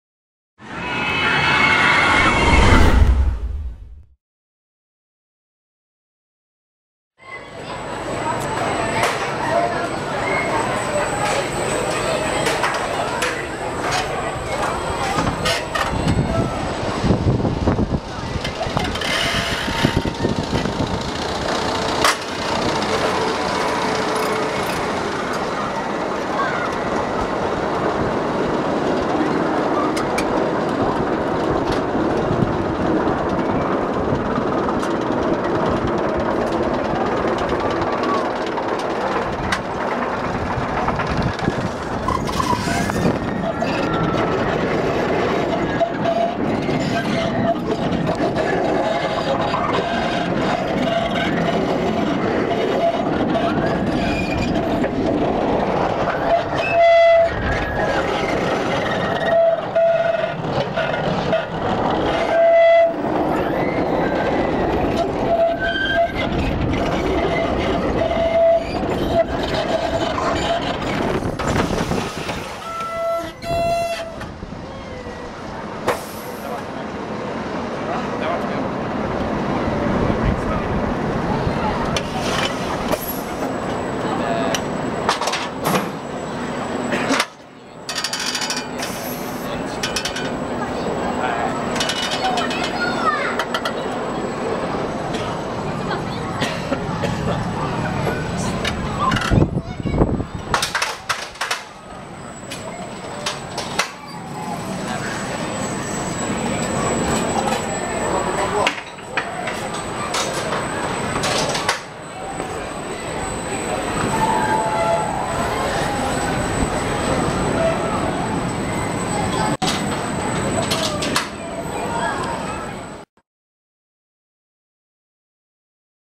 Tranan right side front seat on-ride HD POV Skara Sommarland
This one-of-a-kind S&S Worldwide suspended Free Fly coaster is not only very quirky and unique, but is also surprisingly fun to ride.
If you are confused about exactly what's going on, please check out the left side on-ride ) and the off-ride ) videos we posted to see how it works.
Skara Sommarland is located in Västra Götaland, Sweden.